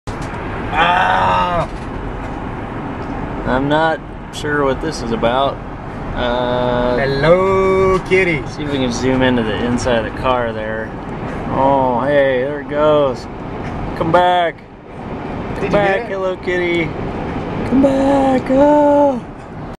The Hello Kitty Car - Orange County
I was driving from LA to Orange County and had to take a video of this car I saw driving beside me. The video doesn't begin to show the glimpse I got of the interior. The exterior and interior of this car is tricked out with Hello Kitty gear. I wouldn't be surprised if the engine parts weren't somehow reconfigured to be Hello Kitty driven.